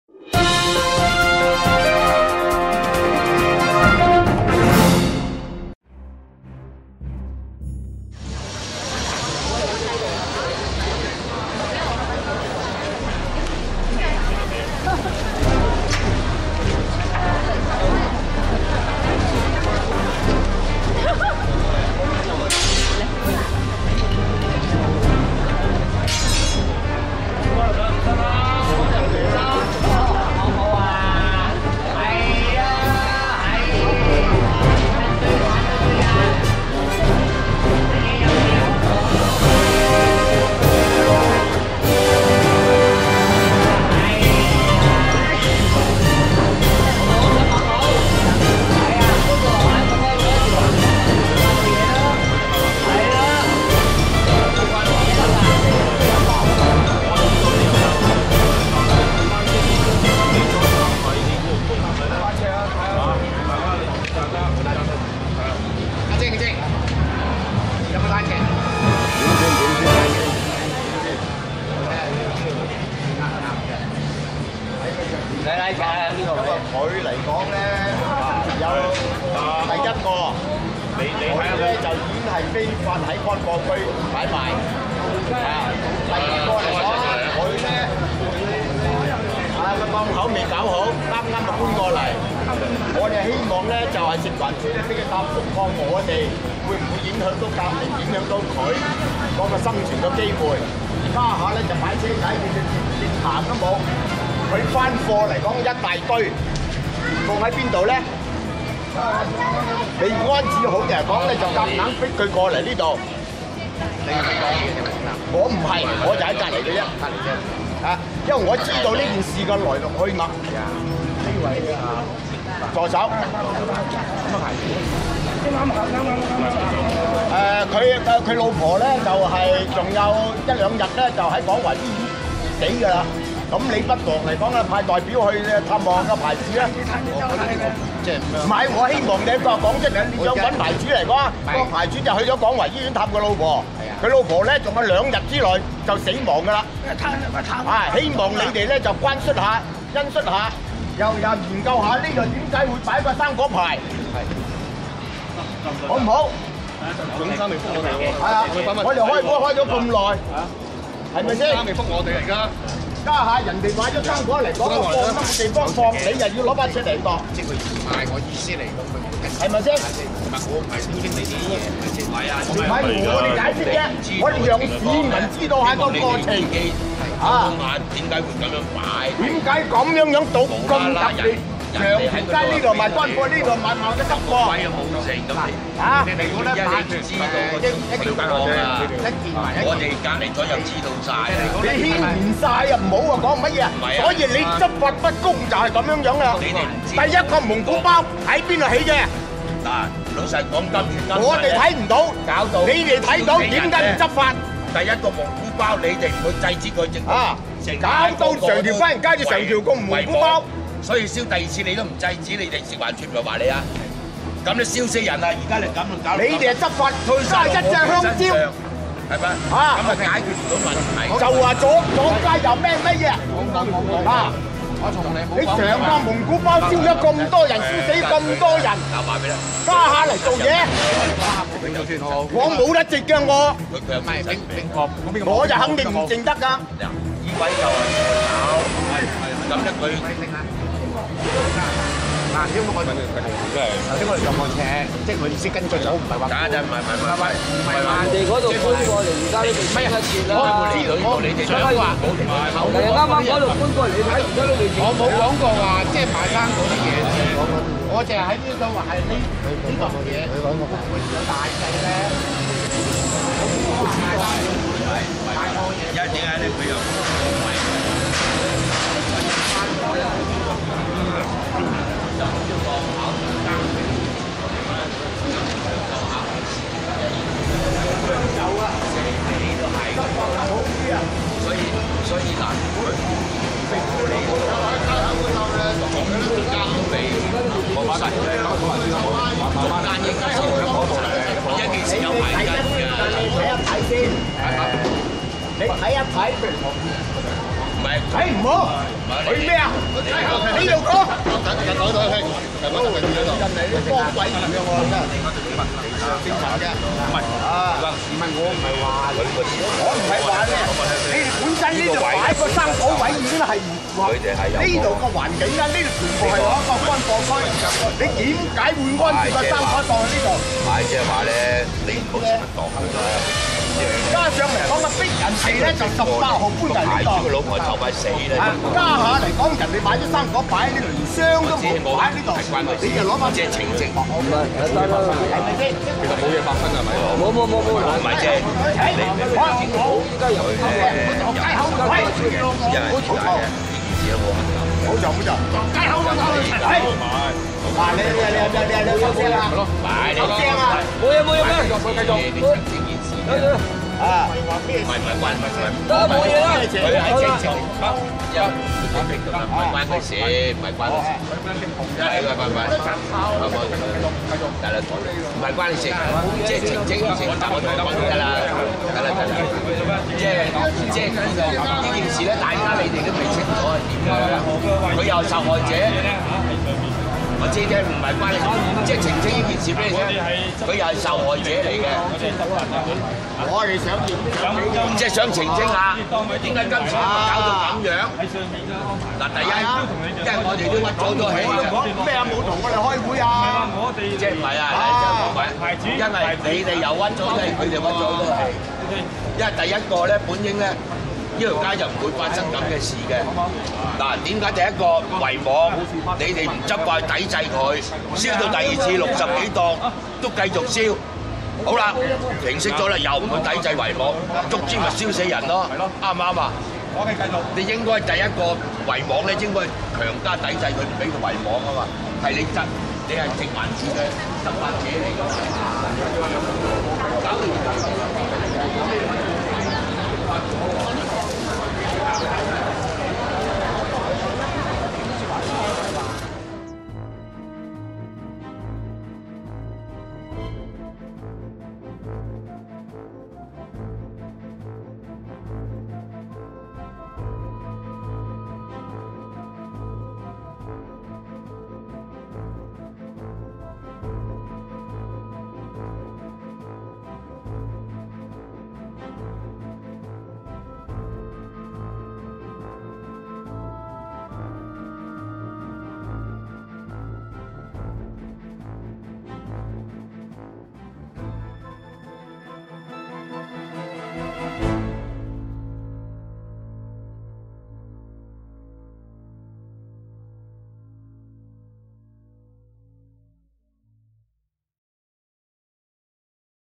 20111216 花園街實錄 (A) 阿Sir真辛苦! [人網制作]
[香港人網記者團隊 HKRNEWSGROUP CHANNEL 的頻道]

☆  低 層 食 環 署 執 法 辛 苦 ！  ☆
☆  低 層 小 販 搵 二 餐 艱 難  ！ ☆
☆ 高 層 政 府 之 父 母 官 轉 移 視 線  ☆
☆  社 會 矛 盾 分 化  責 任 在 誰 呢 ？ ☆
◎　慘 劇 責 任 推 在 同 樣 受 害 的 小 販 公 平 嗎 ？　◎